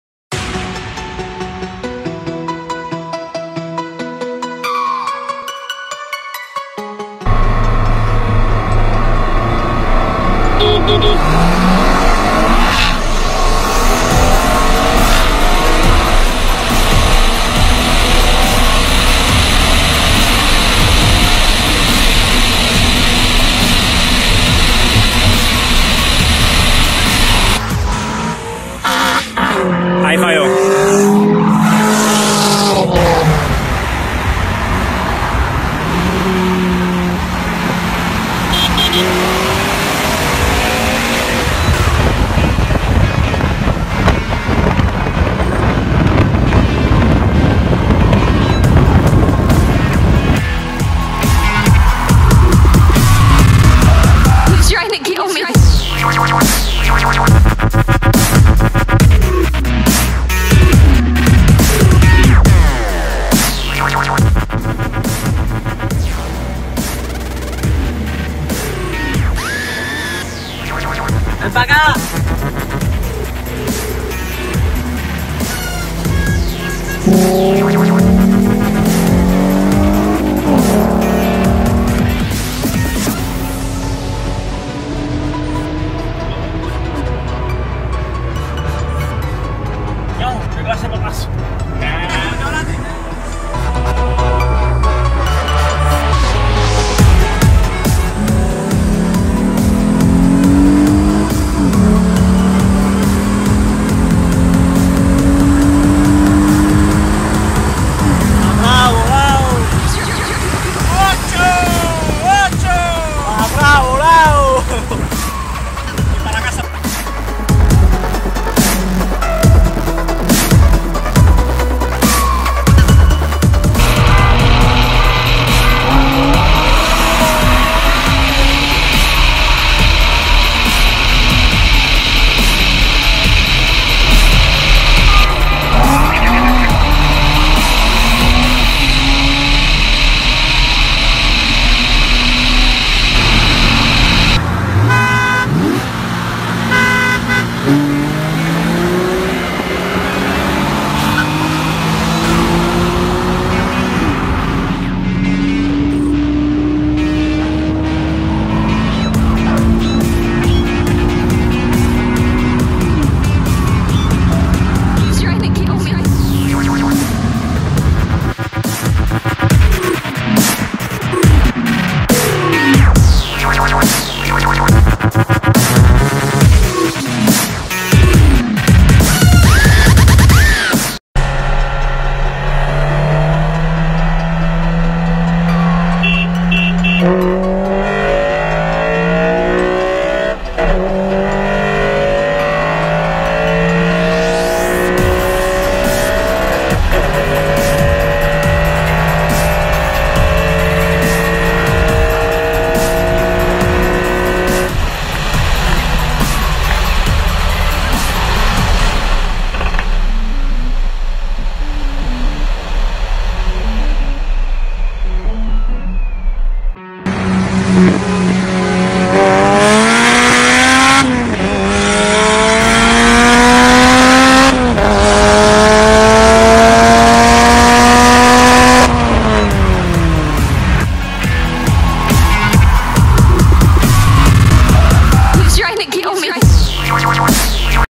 2016 WRX and 370z vs the World plus turbo civic, mustang and more.
Compilation of some newer and older races
procharged v6 mustang
stock v6 mustang
2014 3.7 v6 mustang
Turbo civic si
Toyota Supra
Nissan 370z
Acura RSX
Genesis 4.6 v8
VW GTI